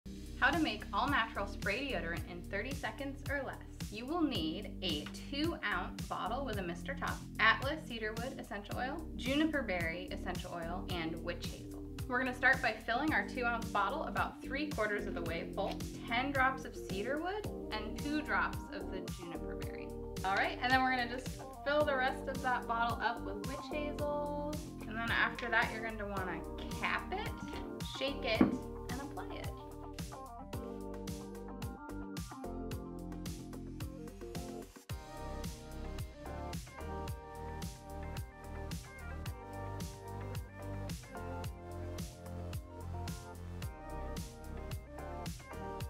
Easy DIY Deodorant Spray Recipe with Cedarwood & Juniper
Pure organic witch hazel extract is a time-tested ingredient in DIY natural deodorant sprays. Distilled from the twigs and bark of the witch hazel tree, the extract lowers the skin’s pH, so odor-causing bacteria cannot thrive. It is gentle enough to be used alone or can be combined with herbs and essential oils, and it also does double-duty as a skin toner.

This homemade deodorant spray recipe brings together the soothing powers of witch hazel extract with balanced essential oils that make them perfect for just about everybody. And, once you have the knack for making it, you can customize the aromatics to make them your own.

Strengthening the bonds between people and plants for a healthier world.